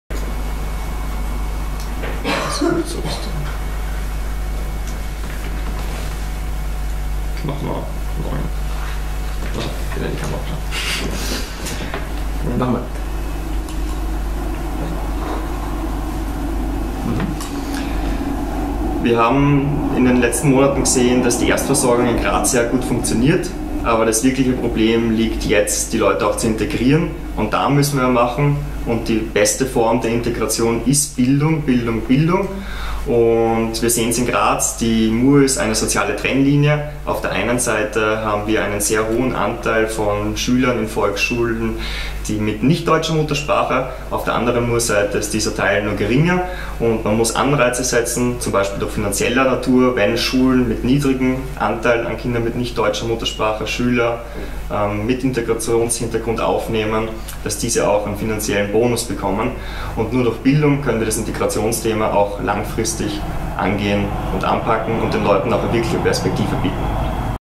5 - Wir haben in Graz genug Platz, um Flüchtlinge aufzunehmen (Niko Swatek, NEOS)
5 - Wir haben in Graz genug Platz, um Flüchtlinge aufzunehmen (Niko Swatek, NEOS)